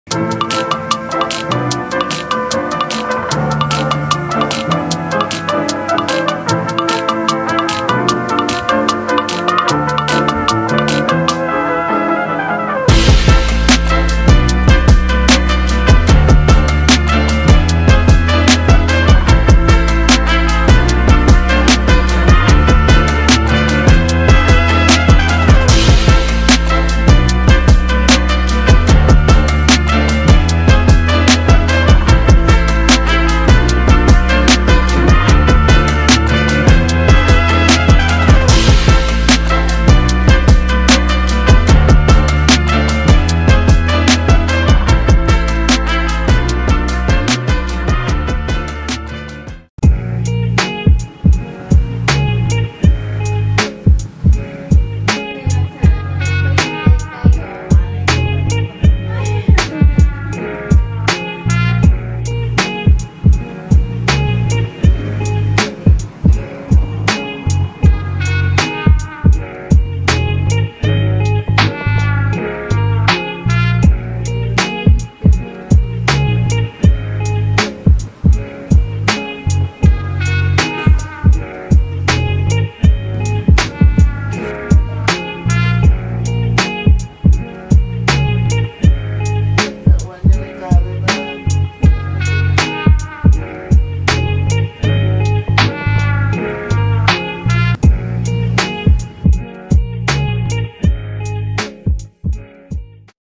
Garland Balloons
Day1 and 2 for these garland balloons i made.
These can served as you reference or guide for color combination of balloons for your future occasion.